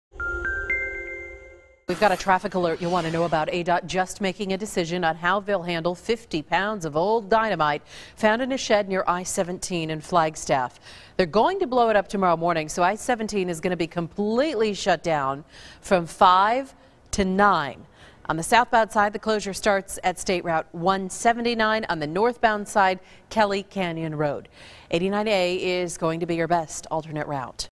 I-17 to be closed tomorrow morning near Flagstaff
DPS is blowing up 50 pounds of old dynamite.